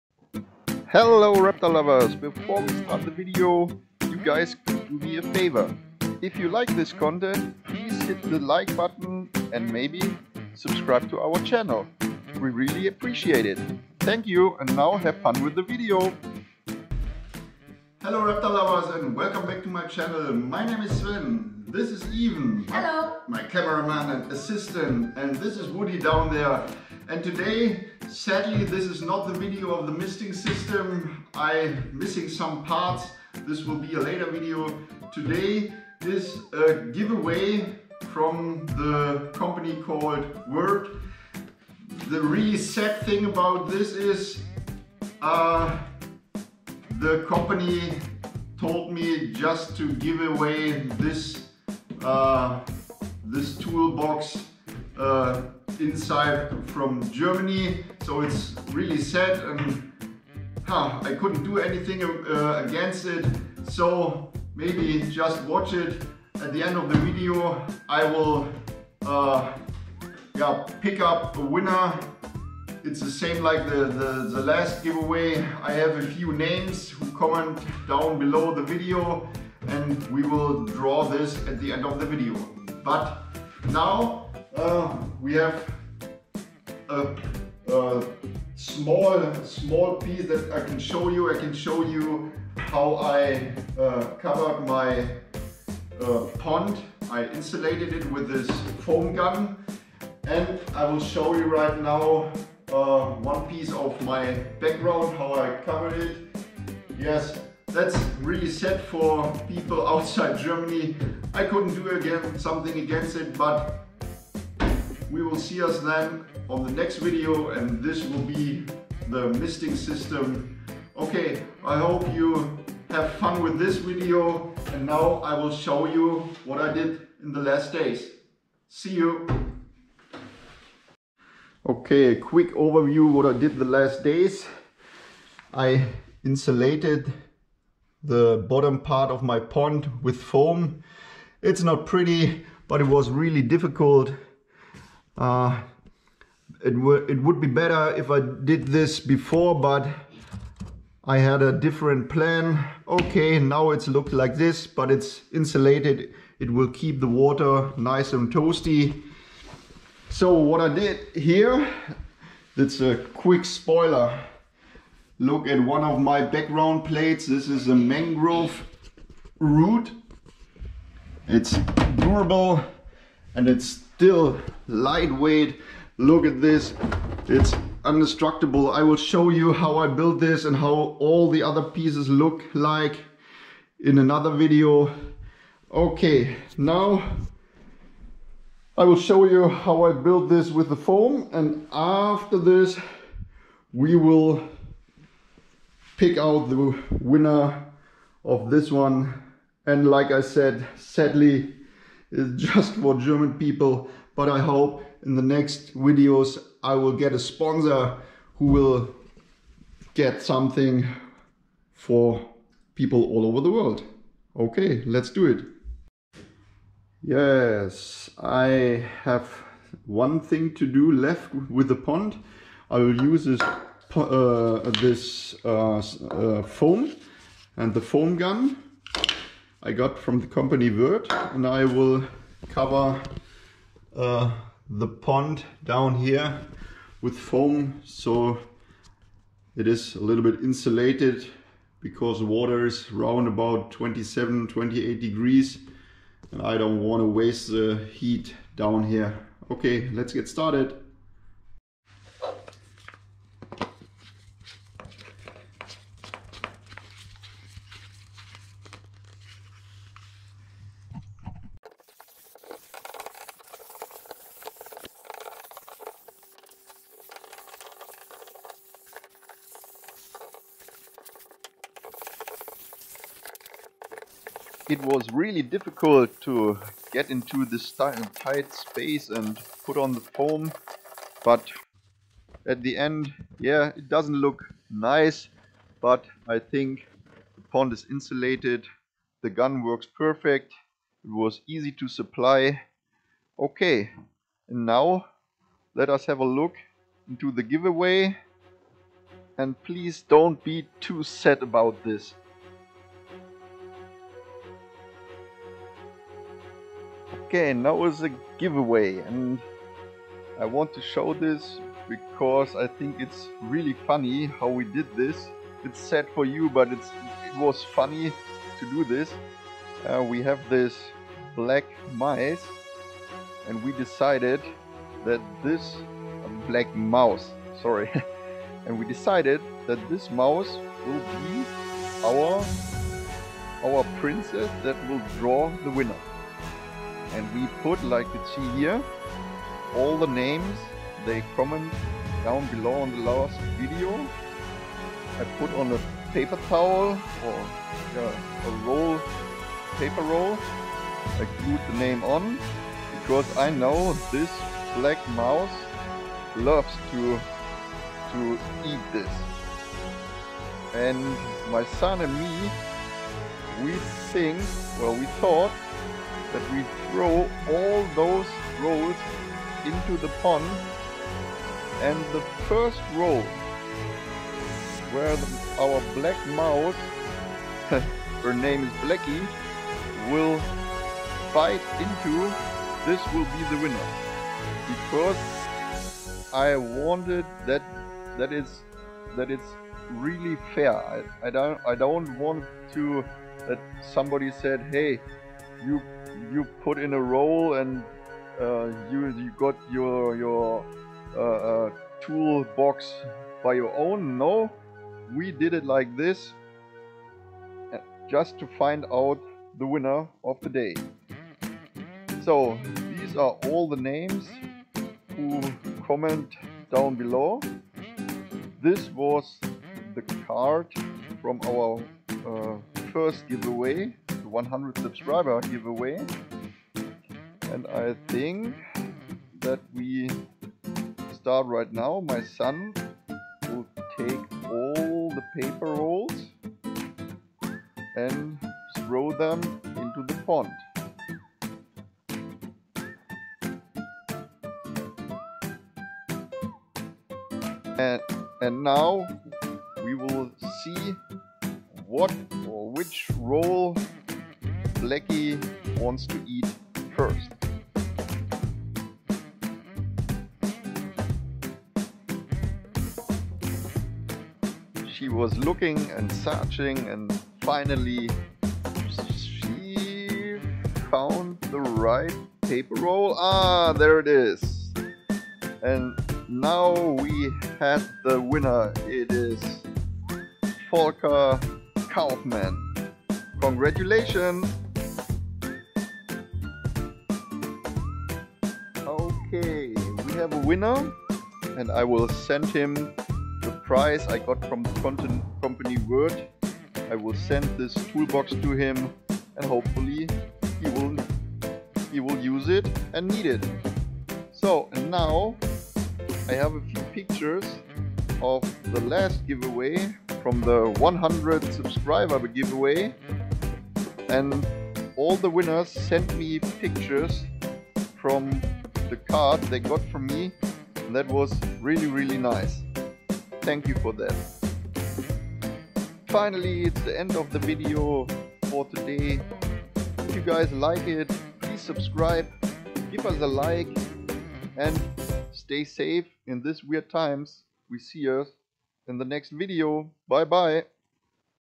Paludarium Terrarium DIY Build series, WÜRTH tool give away, Pond isolation monitor lizard enclosure
Today's Video is all about isolate the pond and a tool give away from the company called WÜRTH. Sadly the company wants the give away only in Germany.
This is my step by step documentation of building an monitor Lizard enclosure, reptile cage or terrarium what ever you want to call it.
The goal is to build an enclosure that looks like part of the nature where the monitor came from.
A few years ago i kept varanus exanthematicus, varanus niloticus and varanus ornatus in my garage and outdoor enclosure.
Now after so many years without lizards I start again and build an enclosure direct into my house.
Please visit me on this exiting journey.

Deutsch:
Dies ist eine Schritt für Schritt Baudokumentation eines Terrariums/ Paludariums als Raumteiler. Einziehen wird in ferner Zukunft eine Waranart die aber noch nicht genau fest steht.
Früher gehaltene Warane waren Steppenwaran varanus exanthematicus, Nilwaran varanus niloticus und Ornatwaran varanus ornatus. Das ganze in Aluminiumbauweise und 3 Seiten Verglasung. Die Beleuchtung kommt von der Firma Econlux Solar Raptor